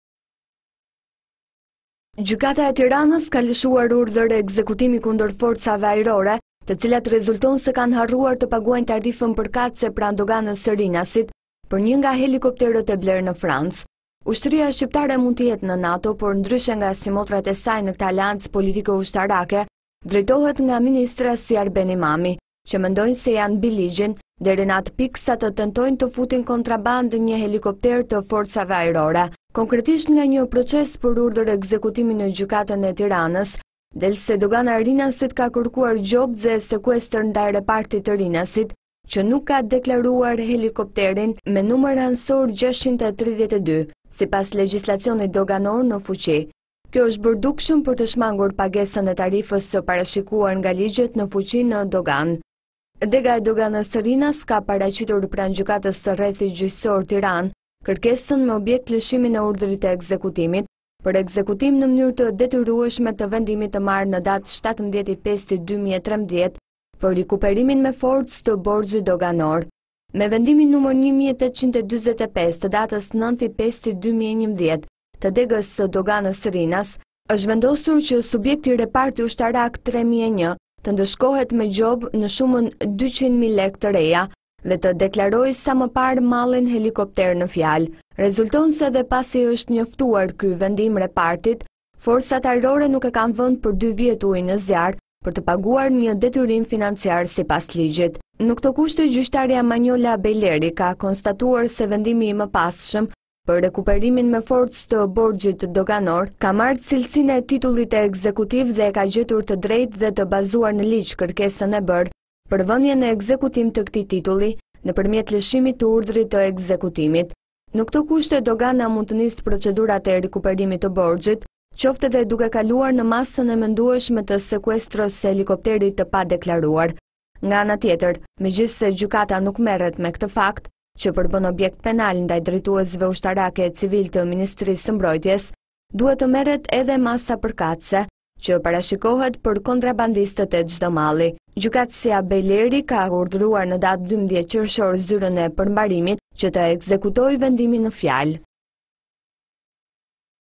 Kontrabandë helikopterësh, ushtria e NATO s'paguan doganën
TIRANE - Gjykata e Tiranës ka lëshuar urdhër ekzekutimi kundër Forcave Ajrore, të cilat rezulton se kanë "harruar" të paguajnë tarifën përkatëse pranë Doganës së Rinasit për një nga helikopterët e blerë në Francë. Ushtria shqiptare mund të jetë në NATO, por, ndryshe nga simotrat e saj në këtë aleancë politiko-ushtarake, drejtohet nga ministra si Arben Imami, që mendojnë se janë mbi ligjin, deri në atë pikë sa të tentojnë të futin kontrabandë një helikopter të Forcave Ajrore. Ndërkohë që nuk gjejnë paratë për një detyrim financiar të padiskutueshëm, shkelin pa iu dridhur qerpiku një ligj tjetër, sipas të cilit 60 ditë para zgjedhjeve asnjë institucion shtetëror nuk mund të publikojë reklama, veça atyre me karakter shpjegues zgjedhor.